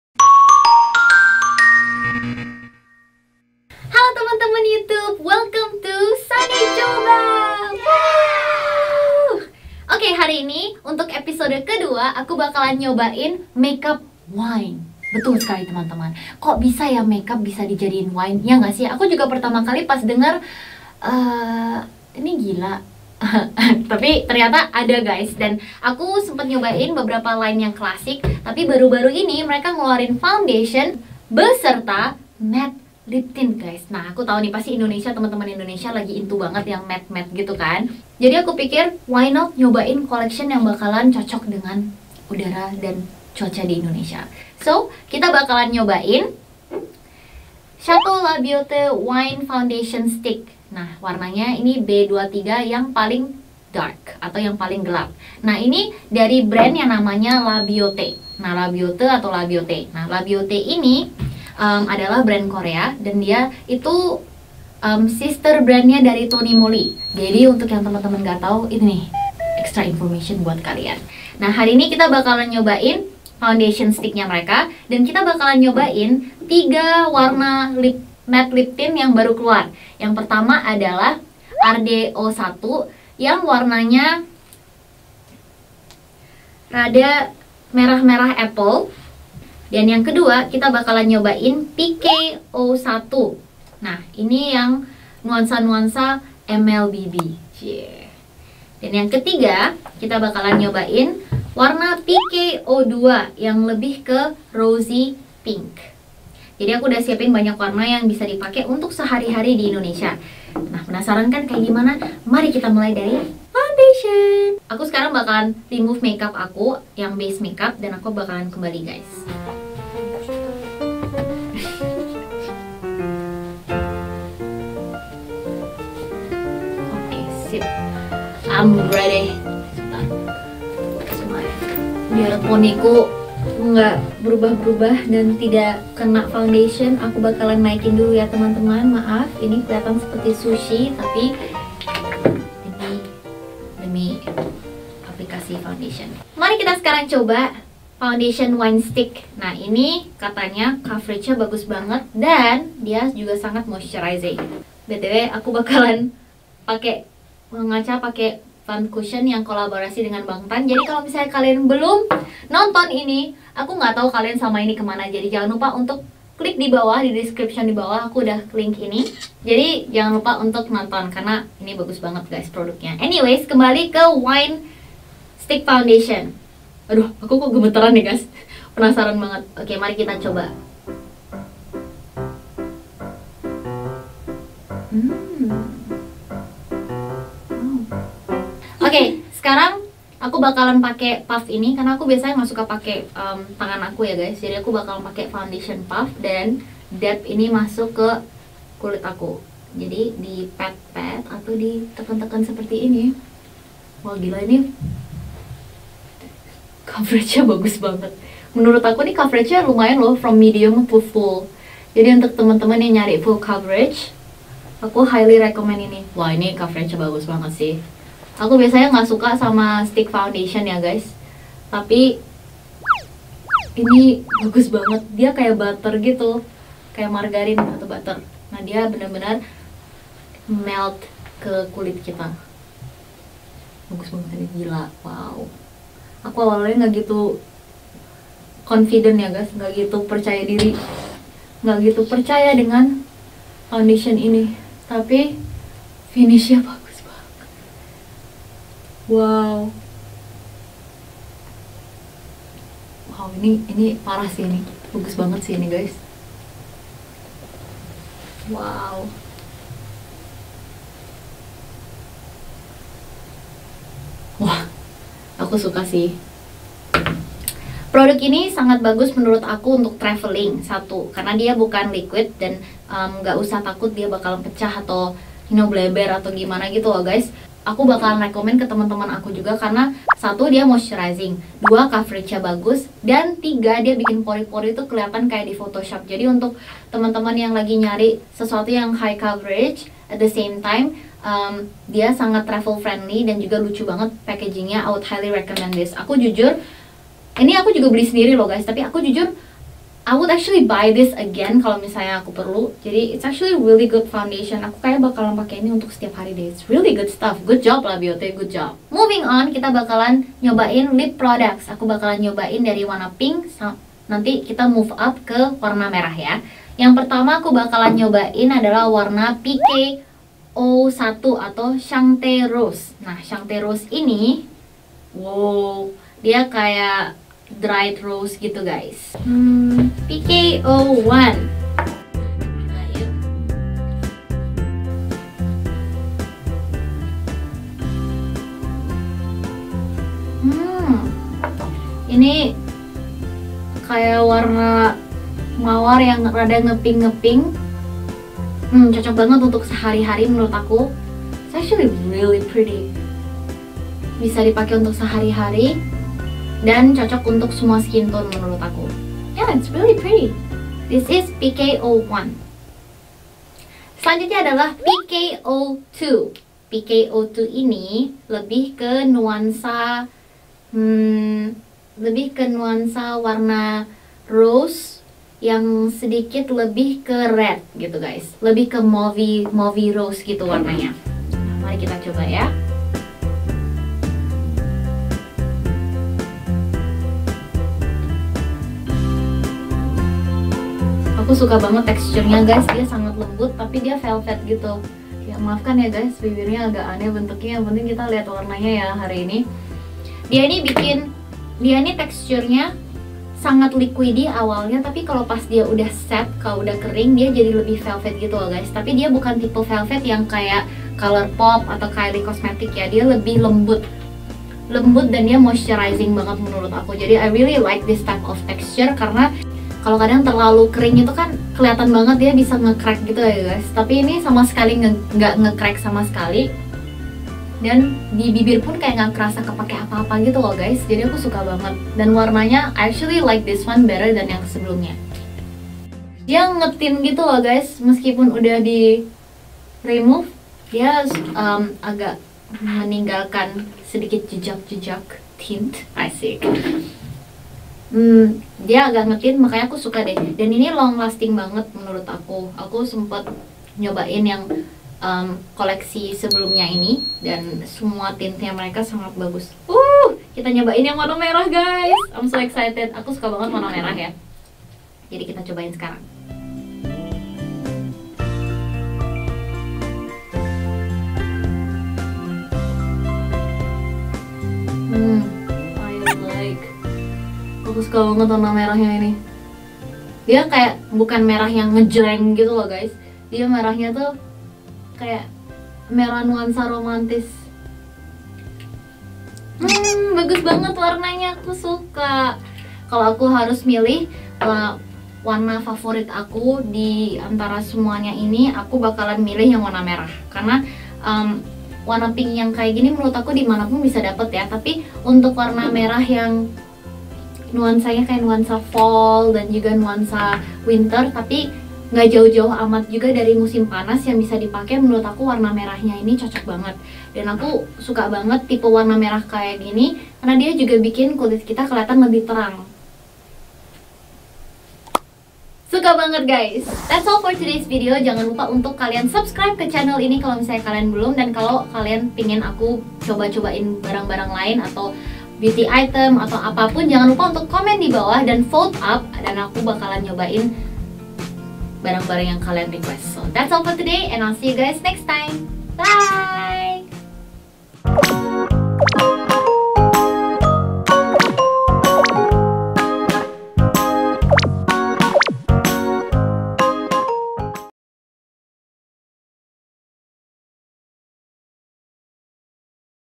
[SunnydahyeIn] 와인으로 착각하면 금물~!! 병모양이 넘나 이쁜 Coba 와인 파운데이션 스틱으로, 와인 메이크업 도전~!!!
[옐리펀트/써니다혜인] 와인으로 착각하면 금물~!! 병모양이 넘나 이쁜 Coba 와인 파운데이션 스틱으로, 와인 메이크업 도전~!!!

크리에이티브 포스 Sunnydahyein의 다른 영상을 보고 싶다면?

더 많은 크리에이티브포스 SBA 영상을 보고 싶다면?
(서울시-SBA가 새롭게 론칭한 1인미디어 채널입니다.)